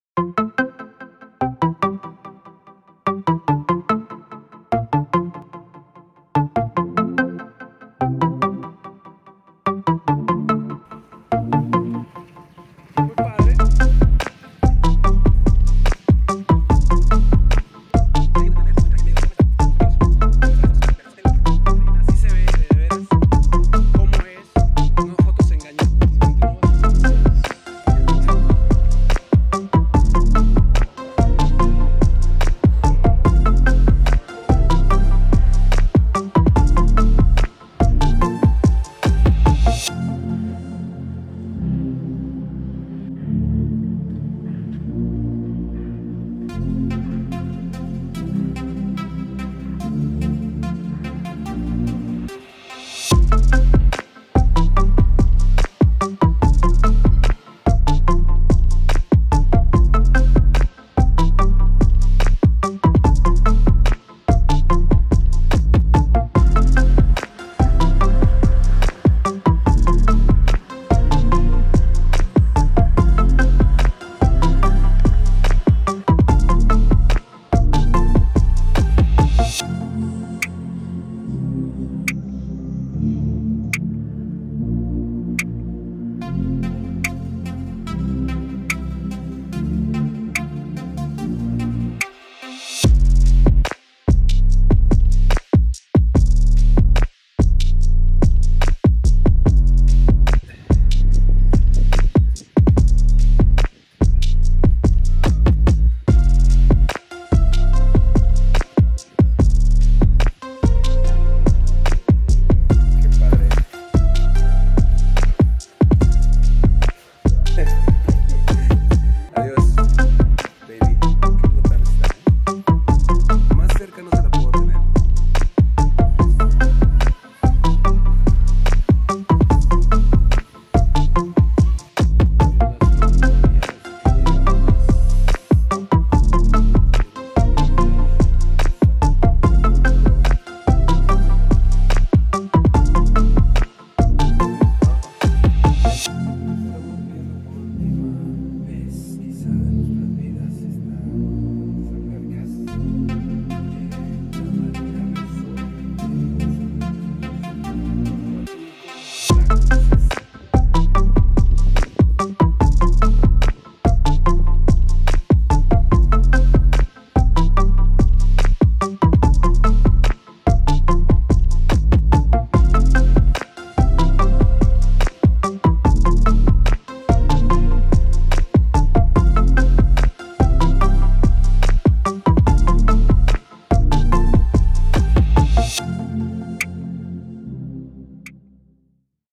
CORTO 07 UNA MIRADA AL AREA DE ALBERCAS DEL MARINA RESORT HOTEL EN HUATULCO OAXACA FEB 2022
Tomé muy pocos videos de este resort. Pero lo poco que tomé, aquí está; para darles una ides de como se ve el area de albercas.
No hospedamos en al habitación 614, estas son las vistas.